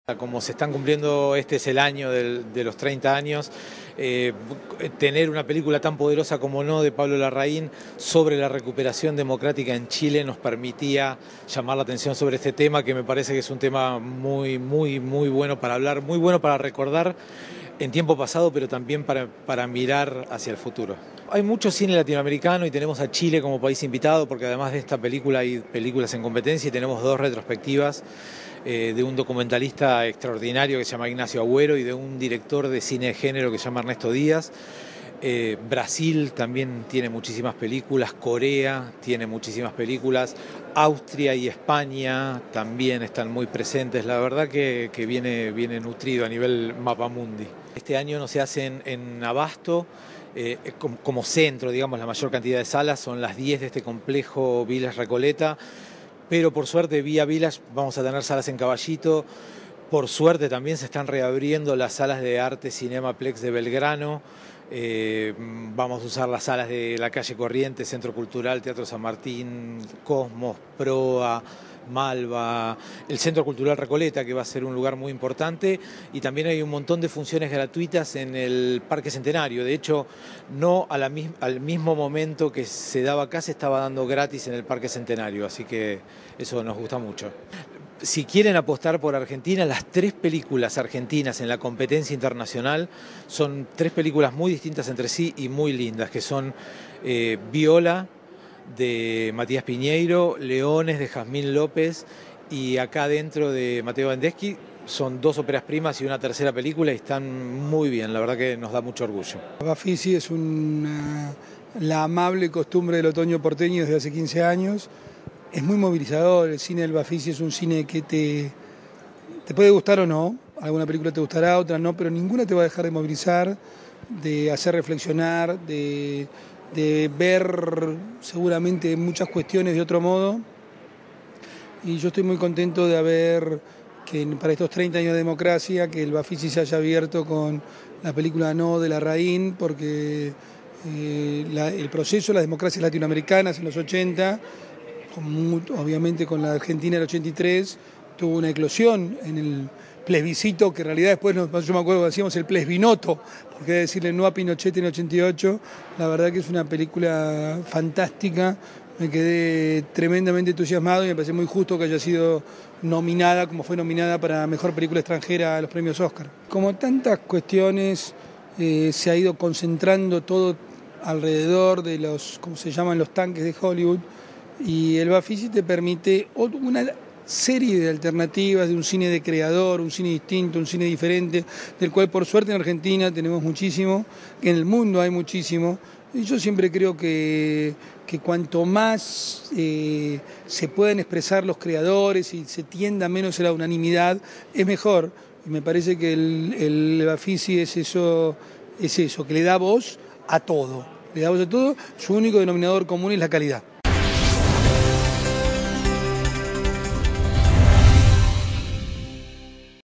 Película sobre vuelta de la democracia en Chile en apertura doble del Bafici
La película chilena No, de Pablo Larraín, que evoca la campaña que en 1988 abrió la
posibilidad de la vuelta a la democracia en Chile votando en contra del dictador Augusto Pinochet, inauguró una nueva edición del Buenos Aires Festival Internacional de Cine Independiente (Bafici), que celebró sus 15 años con una apertura doble en Recoleta y Parque Centenario.

Al mismo tiempo que la inauguración oficial se llevaba a cabo en el Village Recoleta, una de las nuevas sedes del festival, con la presencia de su director artístico, Marcelo Panozzo, el secretario de Cultura porteño, Hernán Lombardi, cineastas, periodistas y otros invitados, en el anfiteatro del Parque Centenario se habían reunido unas 1.600 personas para ver gratuitamente el filme chileno.